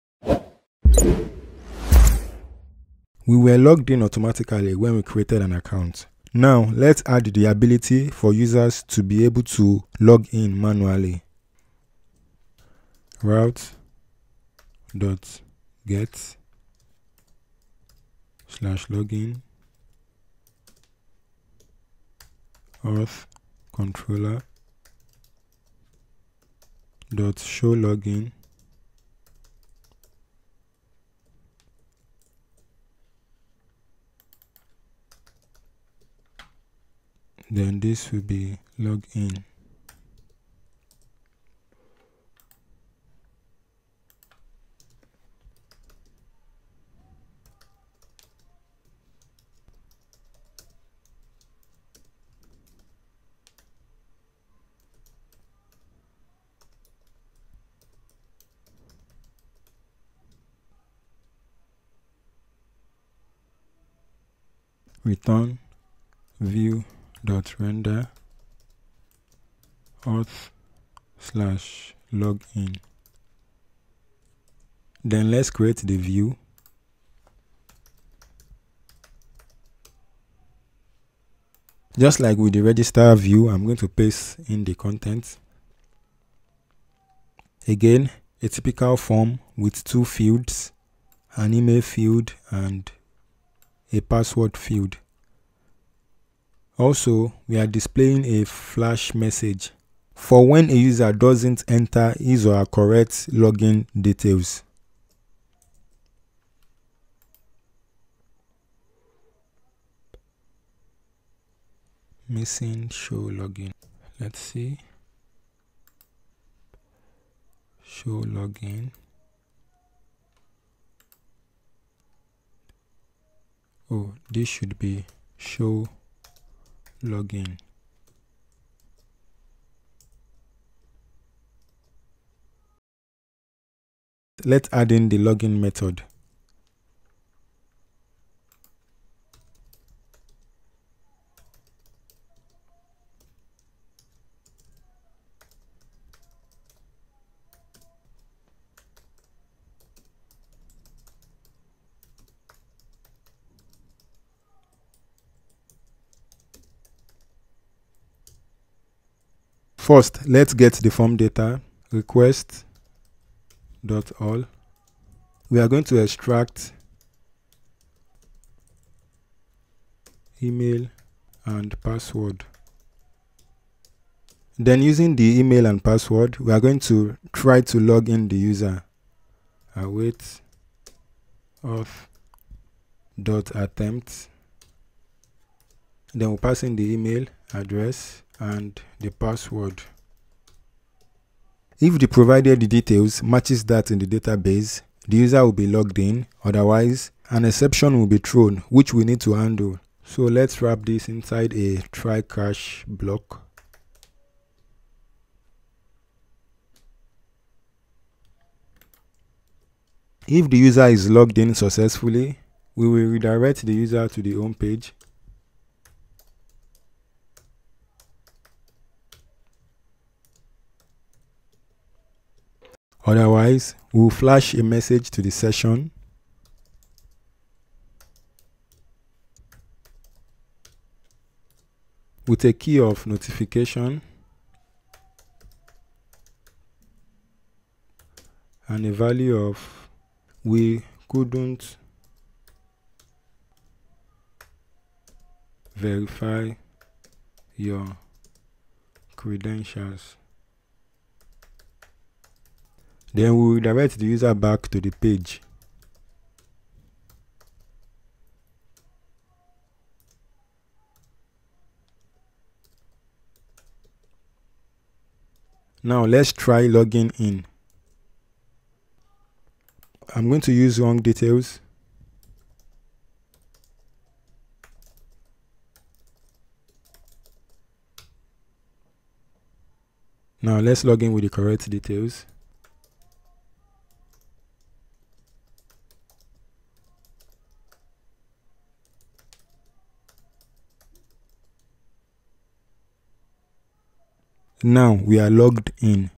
AdonisJS 5 From Scratch #27 - Logging In
Upon registering, users are logged in automatically. In this lesson, we cover how to allow users to manually logging in to their account.